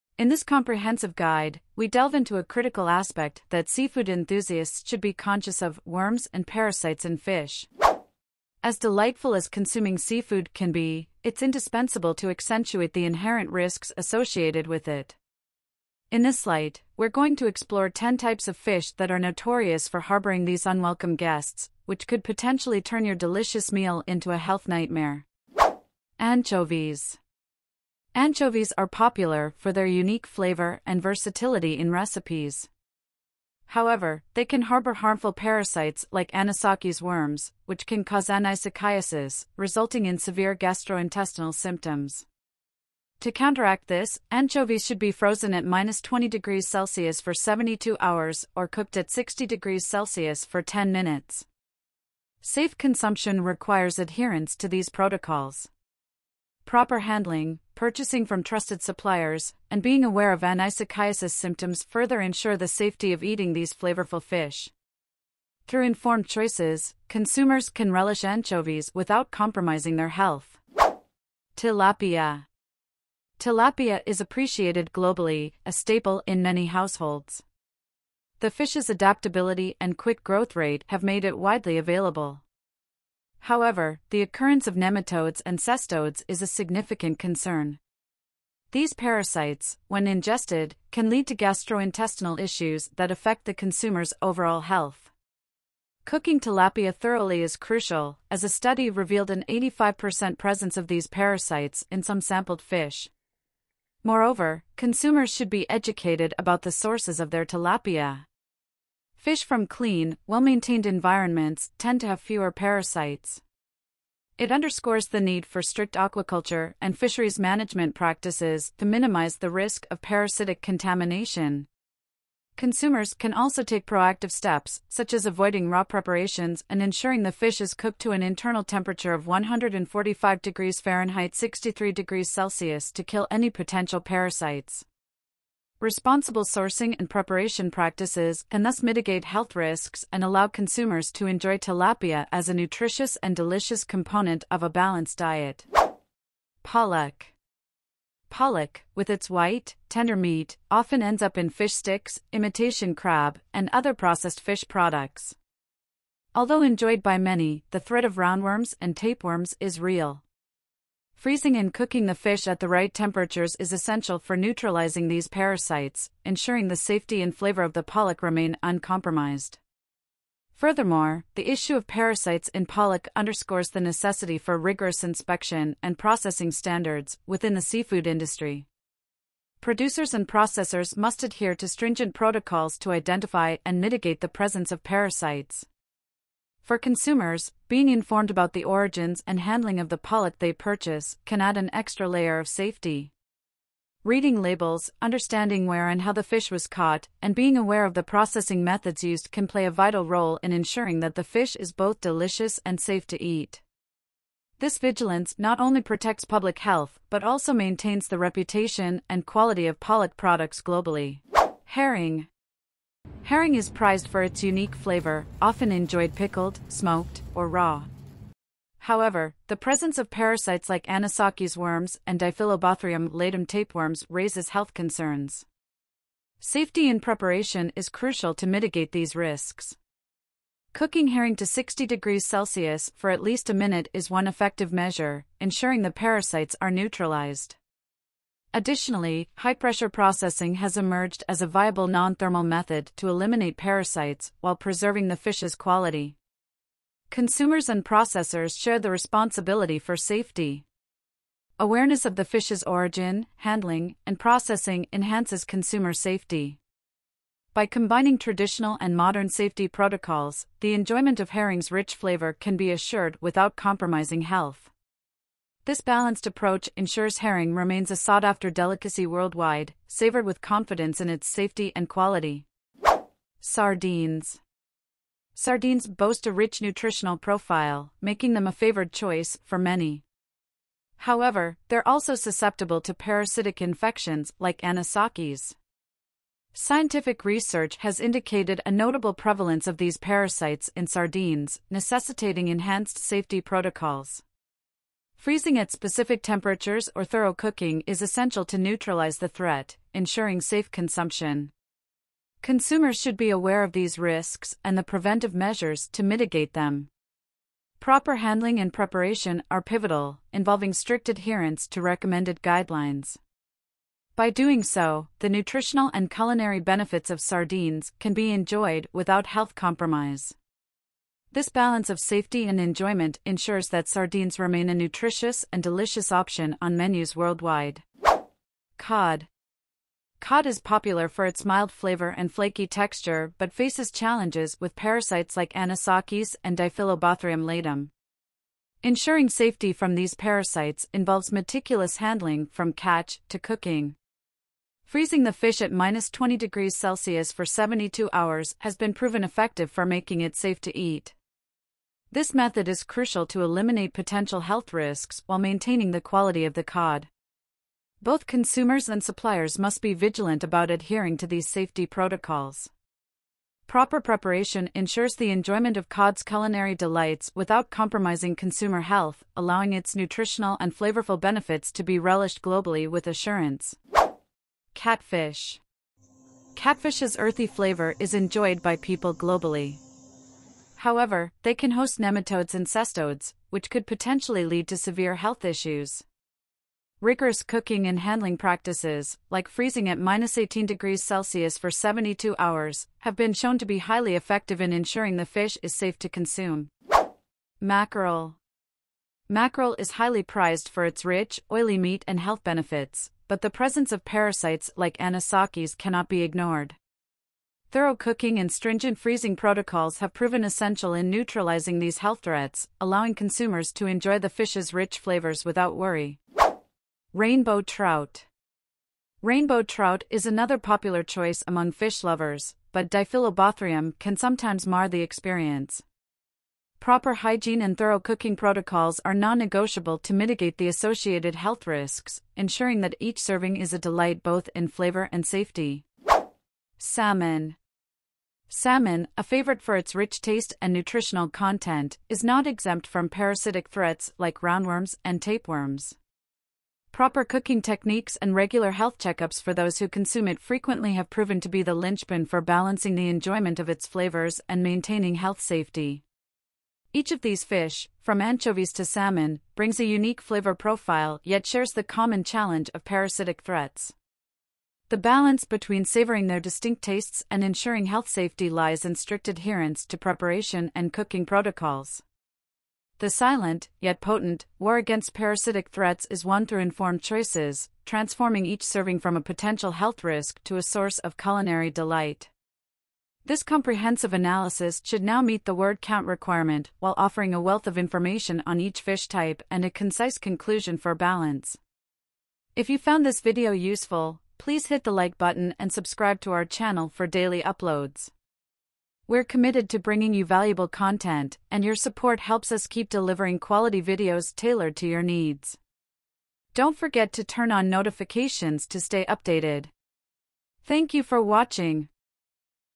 10 Fish that have worms
In this informative video, we dive deep into the world of seafood, specifically focusing on the challenges of parasites found in popular fish varieties like anchovies, tilapia, pollock, herring, sardines, and cod. Discover the unseen world of anisakis worms, nematodes, cestodes, and Diphyllobothrium latum, learning how they can impact your health and how to effectively mitigate the risks associated with them.

We unravel scientific studies and expert insights to arm you with knowledge on safe preparation methods, including specific freezing and cooking temperatures essential for eliminating these hidden dangers. Beyond that, learn about the role of handling, cleaning, and sourcing in ensuring that the seafood on your plate is not only delicious but also safe.

Every seafood lover should be equipped with this information, turning each seafood dining experience into one that is as safe as it is delightful. Armed with this knowledge, you can continue to explore and enjoy the rich, diverse flavors of the sea with confidence and peace of mind.

Your safety and health are our priority. Dive into this guide, and let’s make every seafood meal safe and sumptuous!